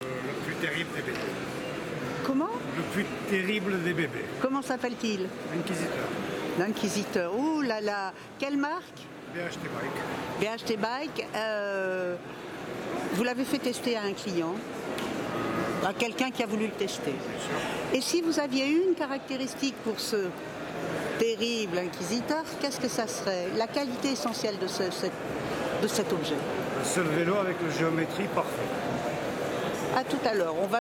Like Bike: l'inquisiteur. BHT Bike. Vélo électrique unique
Like Bike Monaco2016. BHT bike : l’inquisiteur. Un vélo électrique unique qui met touts les autres modèles au défi. confortable et sûr, stable et performant.